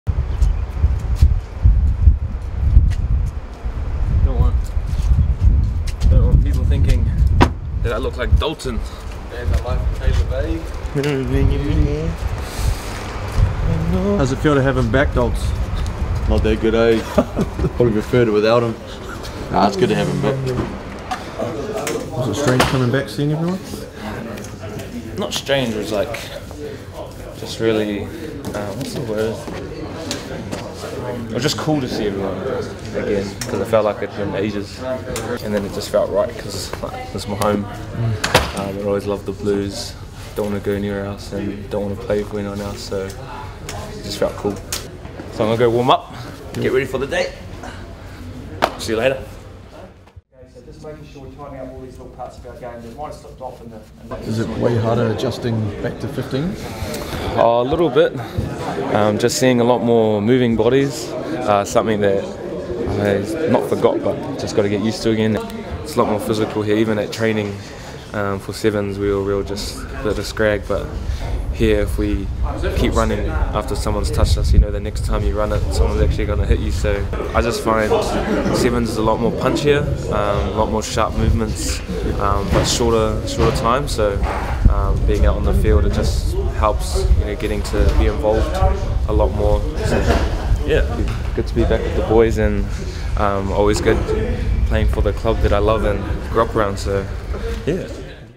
Catch up with Caleb Clarke
Caleb's back from the All Blacks 7's programme! We caught up with him to hear how he's settling back in to the Blues and what it takes to get Super Rugby ready.